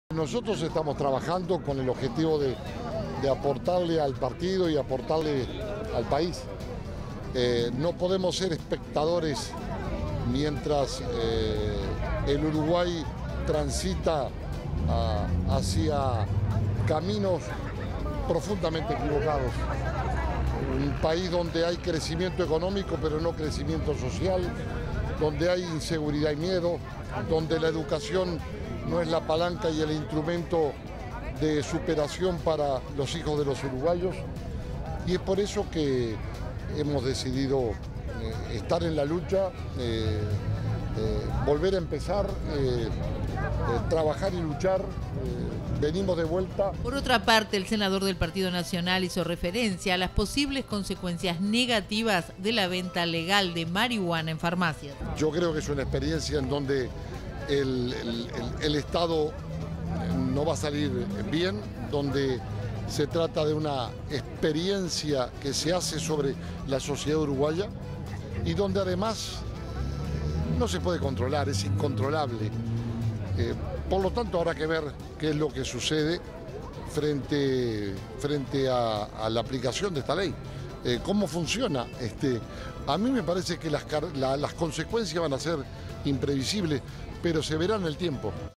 LARRAÑAGA Y SU CANDIDATURA
Jorge Larrañaga dijo que su candidatura será un aporte decisivo para el Partido Nacional, en momentos en que el país transita  por caminos equivocados.